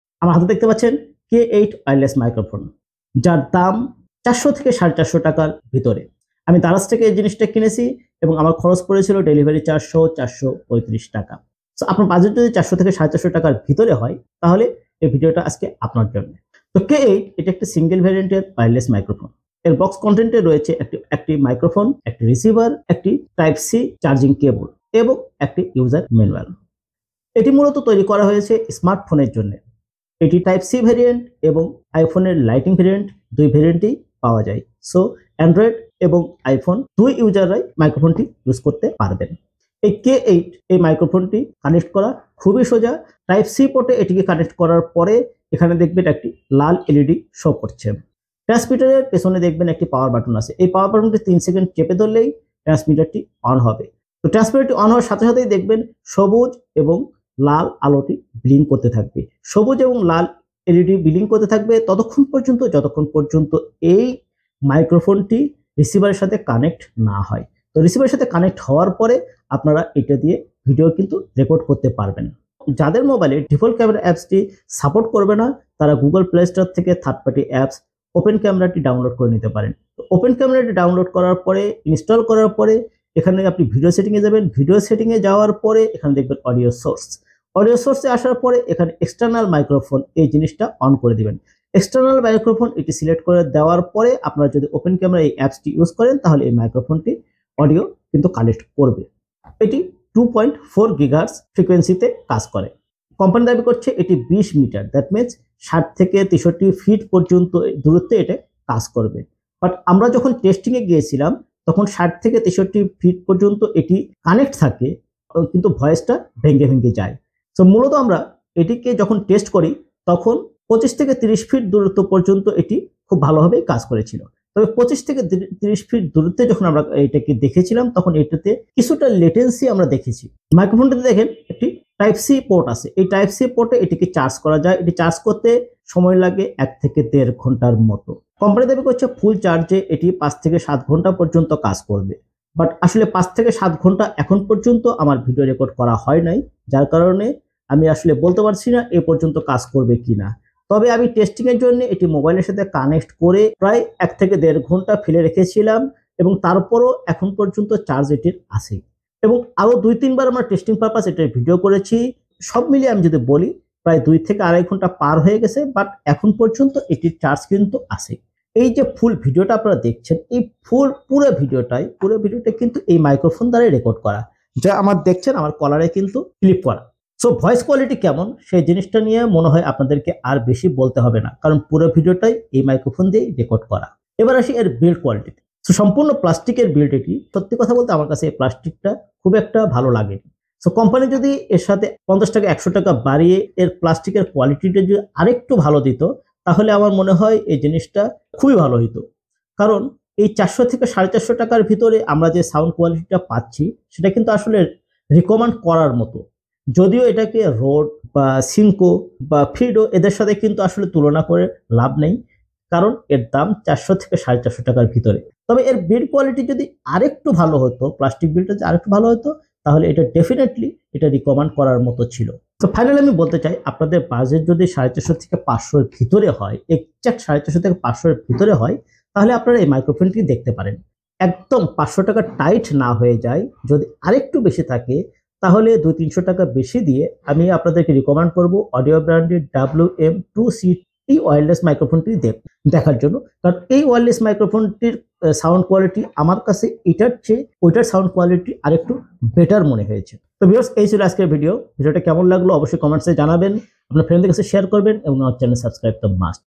নতুন ইউটিউবারদের জন্য মাত্র 435 টাকায় দারুন গ্যাজেট। K8 Wireless Microphone | ওয়ারলেস মাইক্রোফোন
K8 is one of the best wireless microphone for new youtuber. cool gadget only 435 taka. K8 wireless microphone price in BD and K8 mobile microphone review.